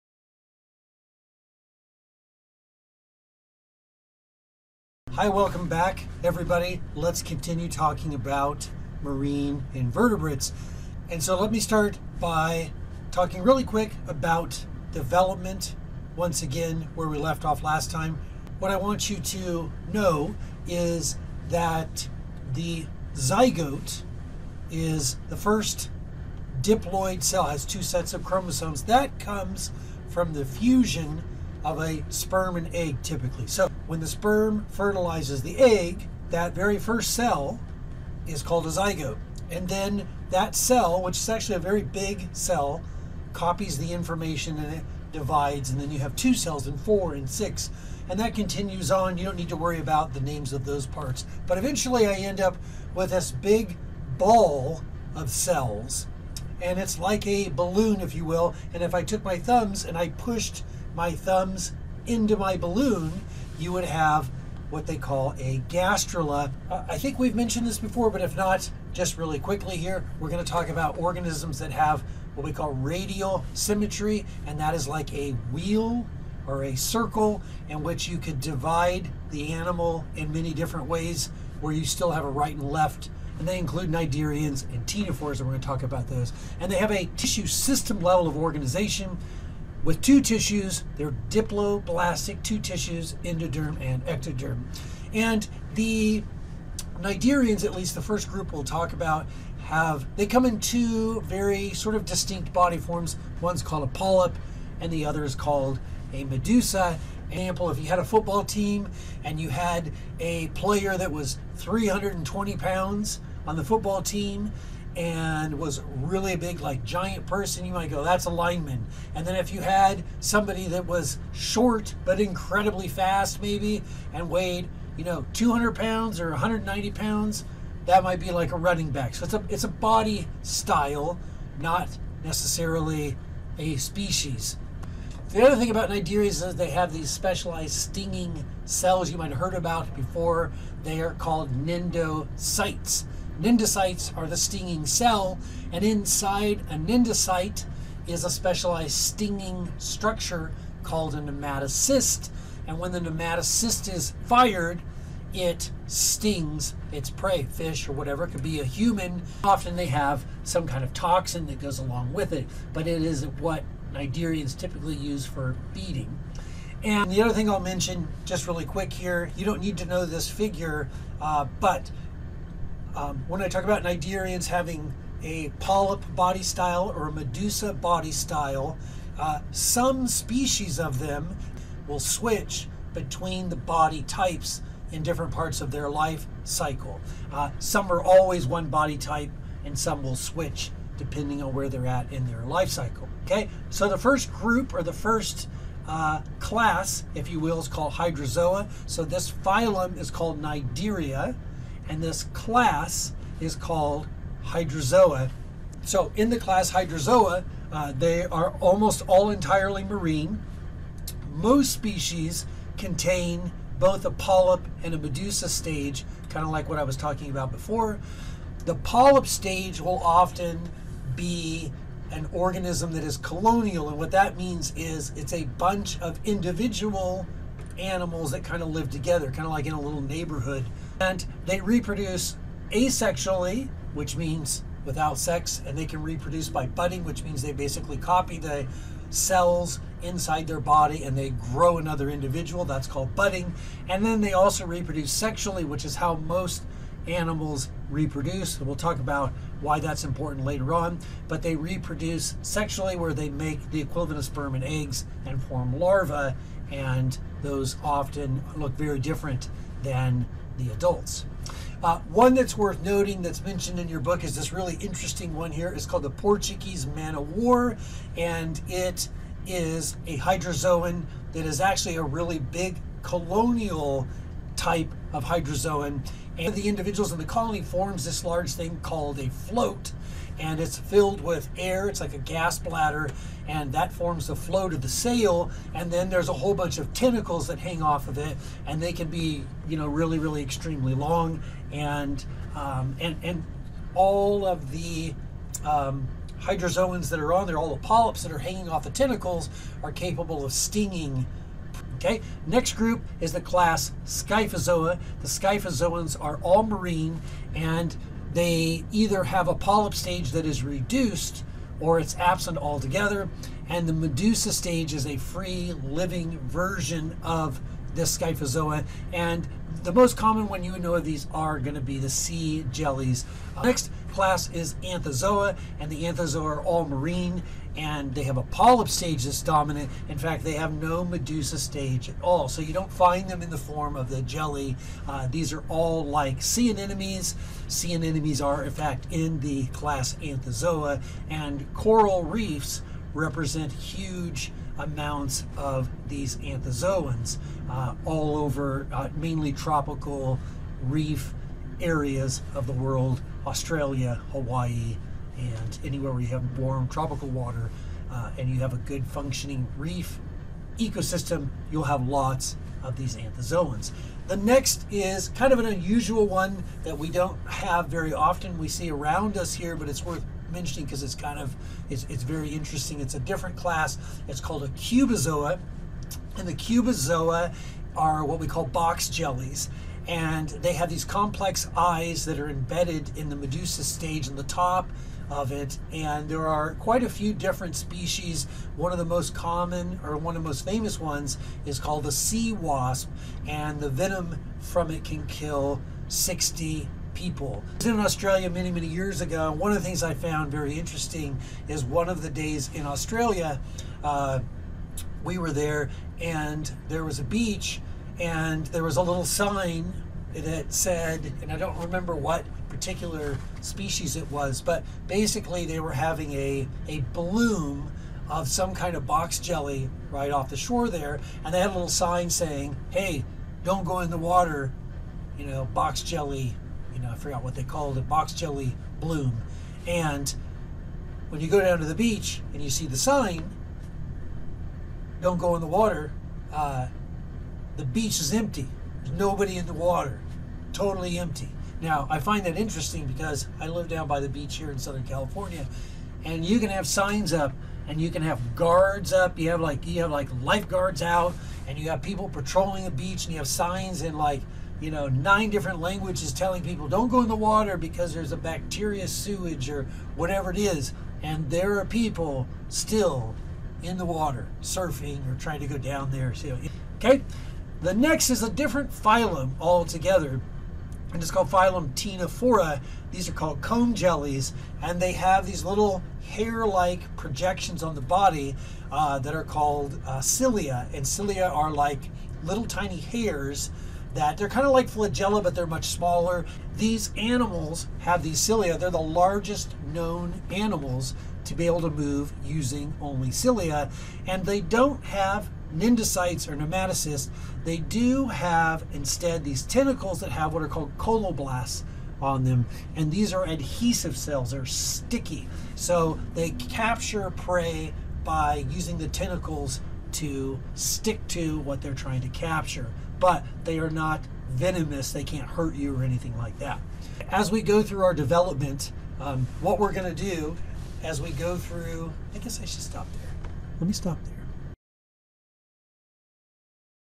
lecture 4 1b inverts 2
This Marine Biology Lecture covers the phyla Cnidaria and Ctenophora